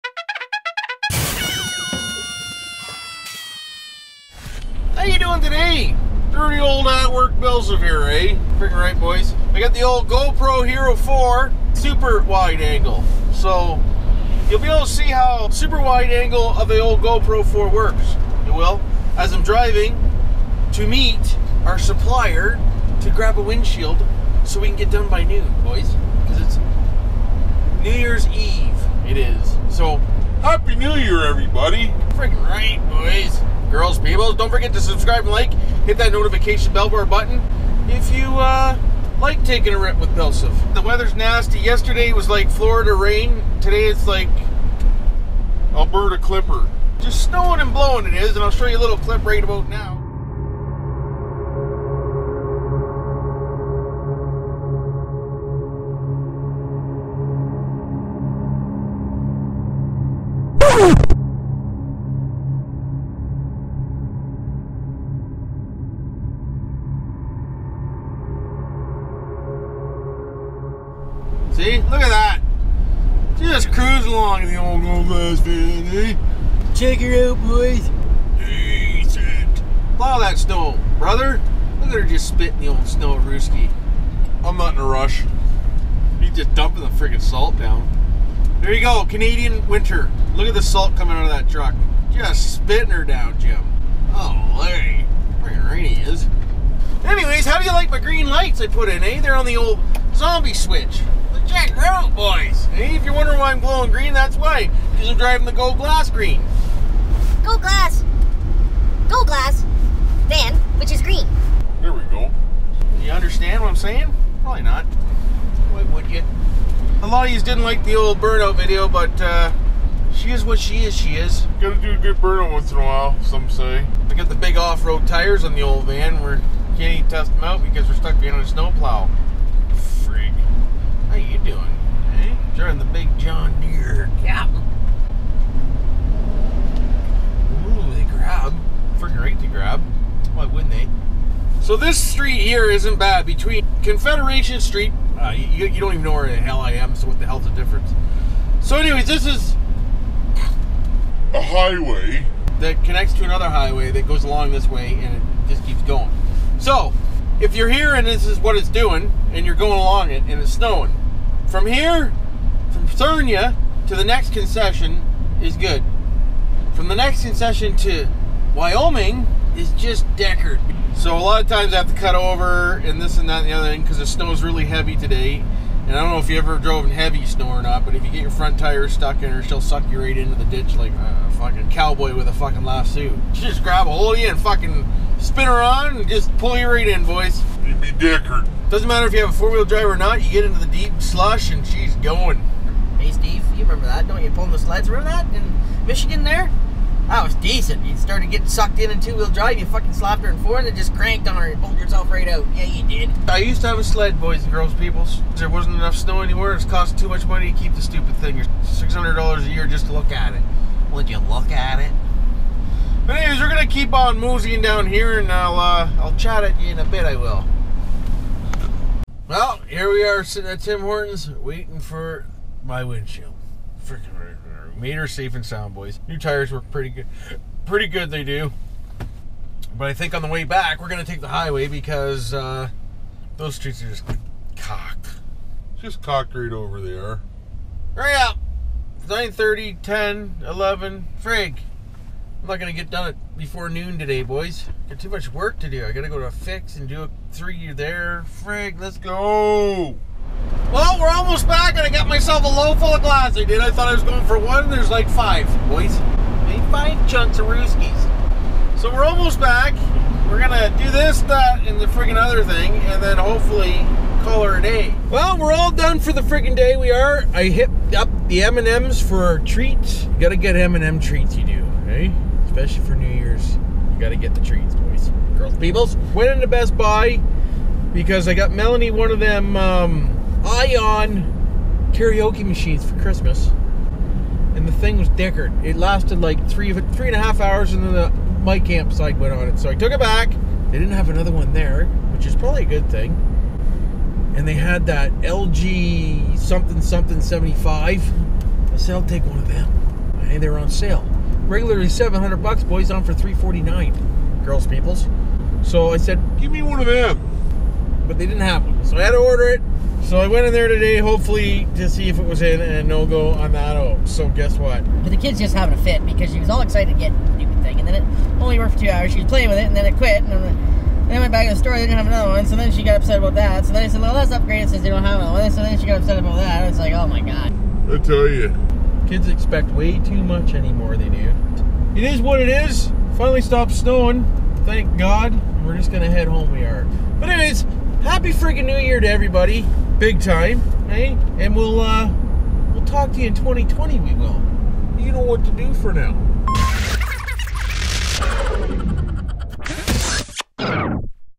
Happy New Year From At Work Bill Siff
Just Like to send Out my love for all of you that support me and my channel and for hanging around with my good and bad video's . I wish only the best for you and your family's through out the year

BillSiff (GoGlass)
1289 Confederation St,
Unit 3
Sarnia Ont.
Canada
N7S 4M7

2540 Lapeer Rd,
Port Huron,